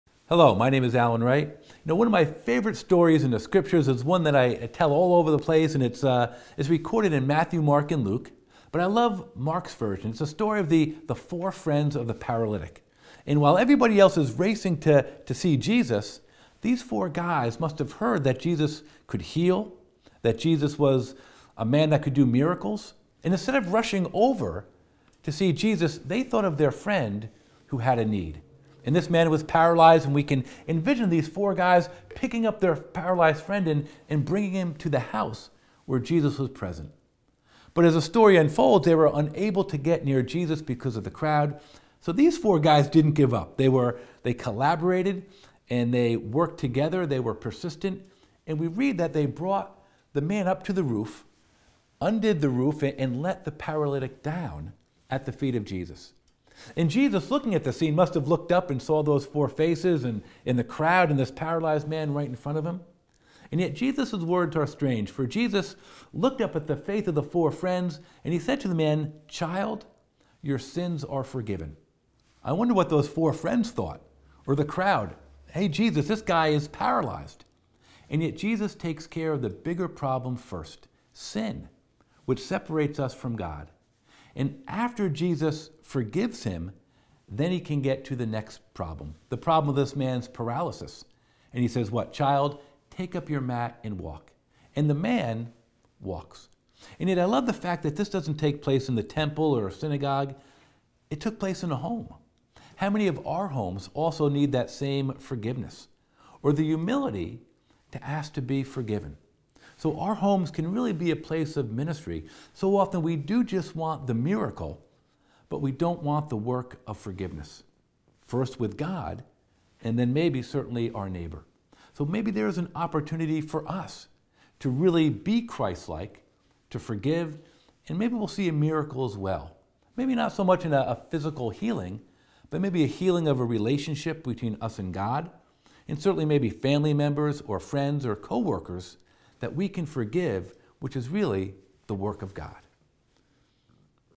Jesus present in the biblical home -4 friends of the paralytic
The word 'home' appears 30 times in the Gospels and the word 'house' appears 99 times. How important is it to have Christ in our homes.